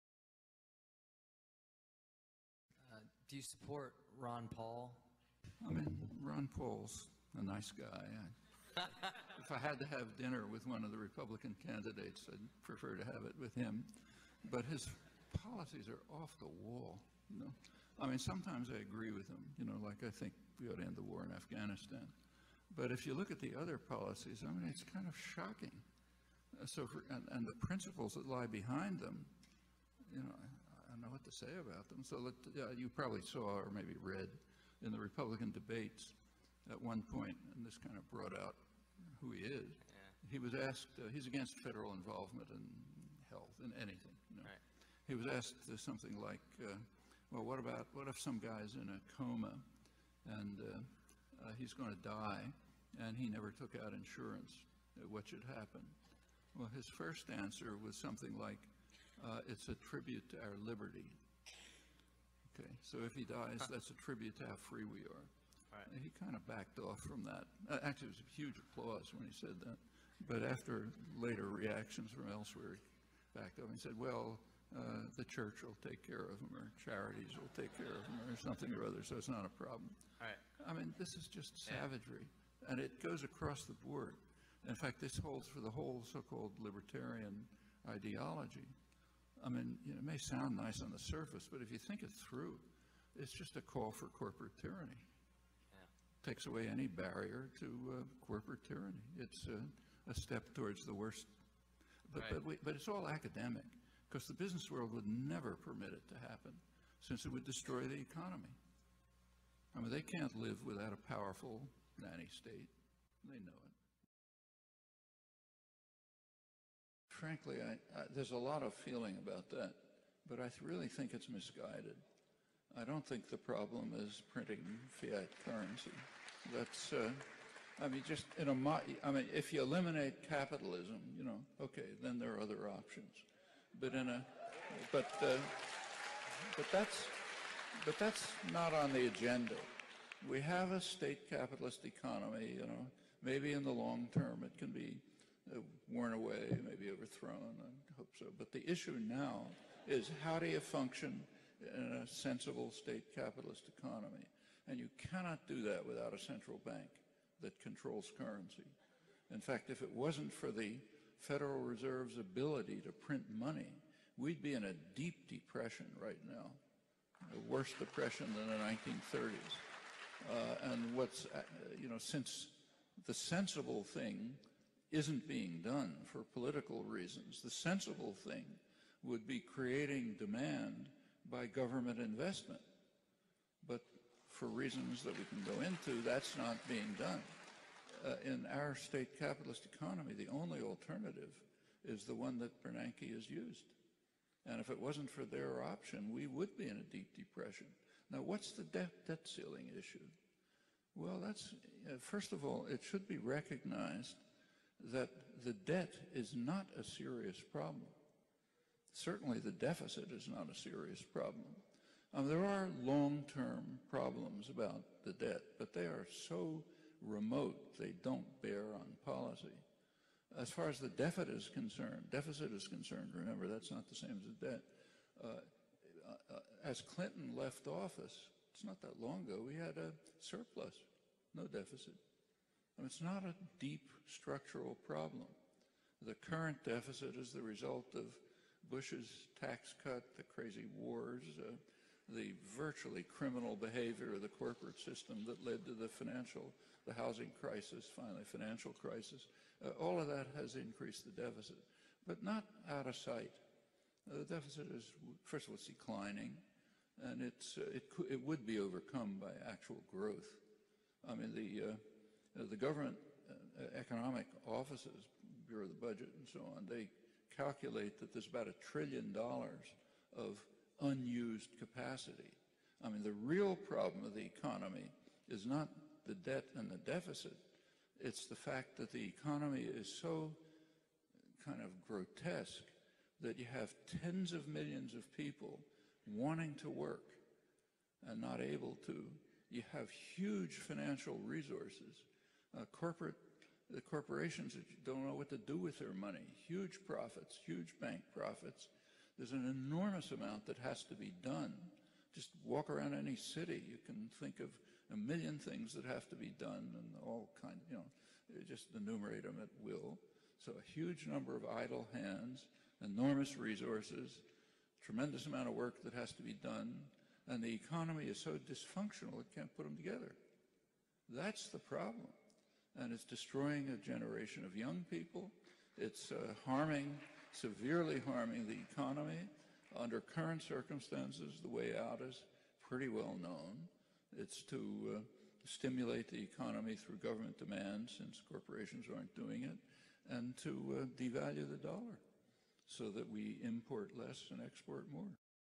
Noam Chomsky refutes right-libertarianism (part 3)
A Call for Corporate Tyranny/The Federal Reserve.